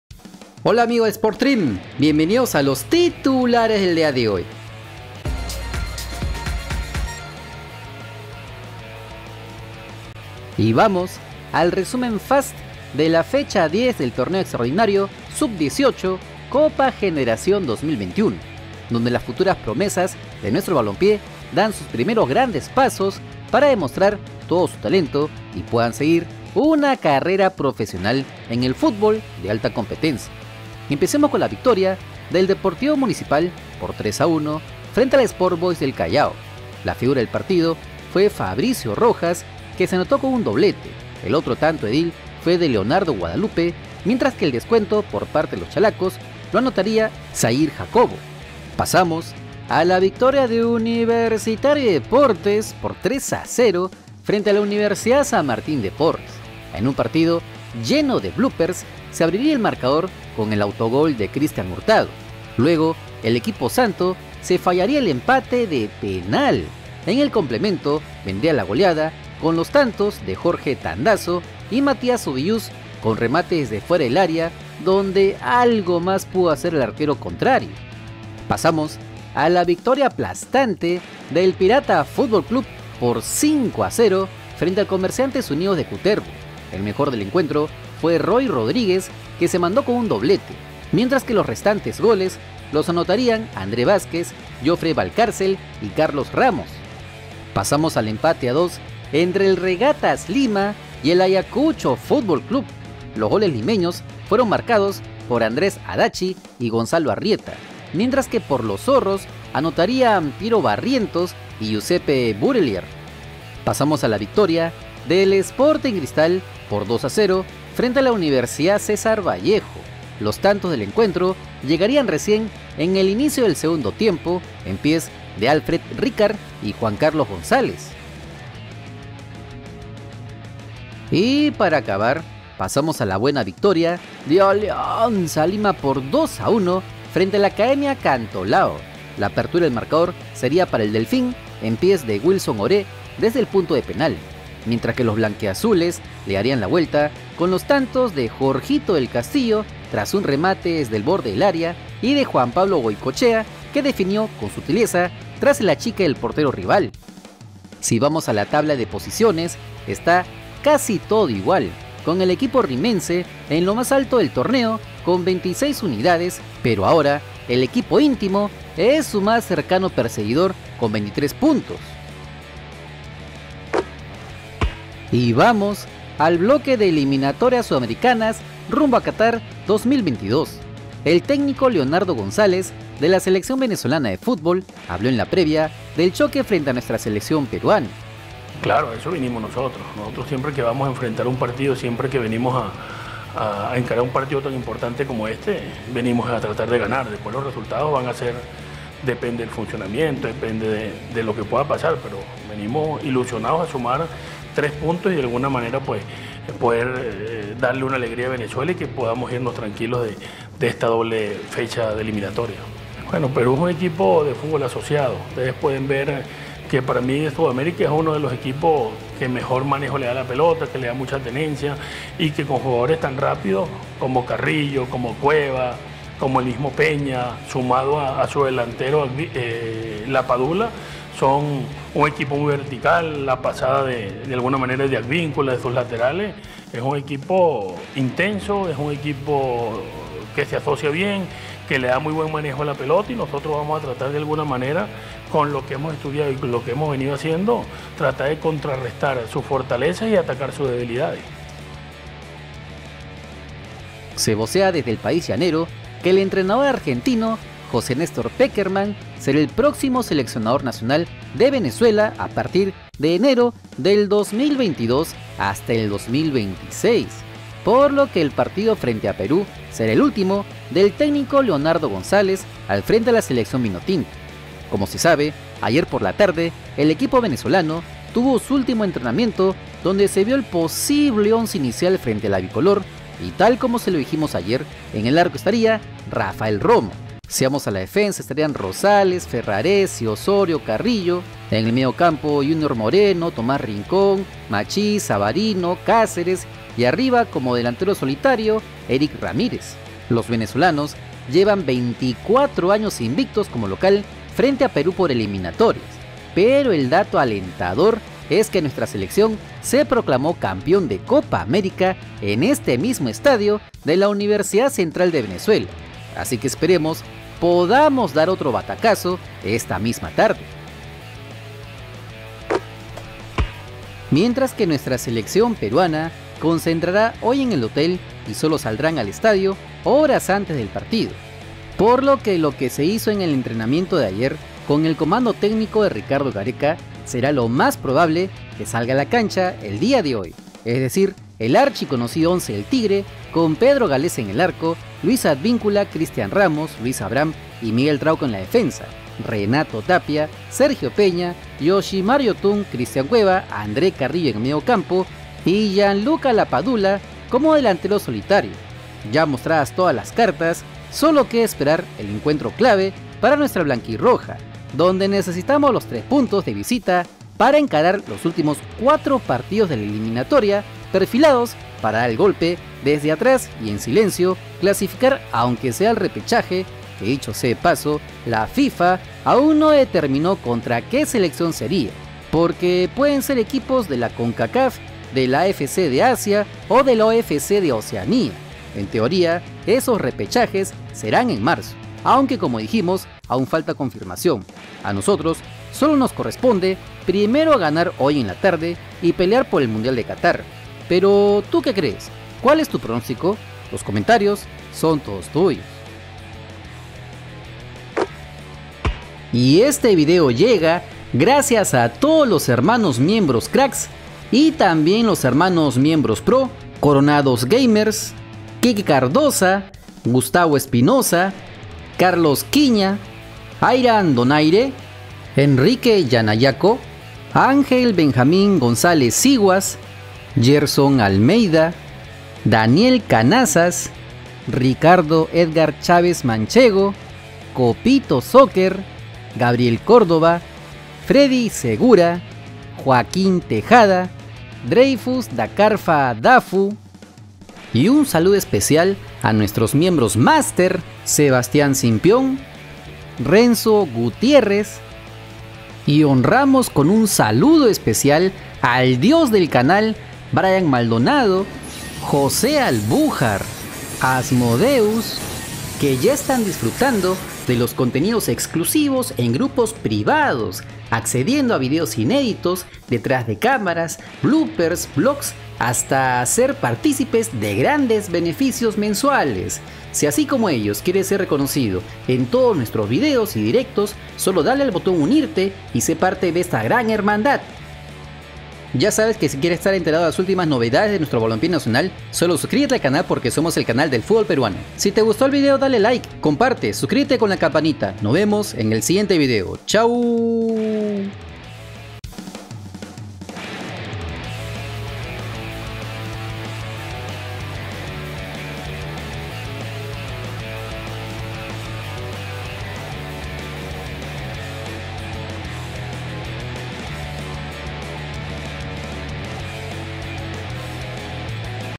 ALINEACIONES CONFIRMADAS de PERÚ v VENEZUELA | DT GONZALES: PERÚ es de los MEJORES | COPA GENERACIÓN
Alineaciones confirmadas de Perú vs Venezuela por Eliminatorias, Técnico Leonardo Gonzales: "Perú es de las mejores selecciones", resumen de la fecha 10 de la Copa Generación 2021 y otras noticias del futbol peruano en titulares.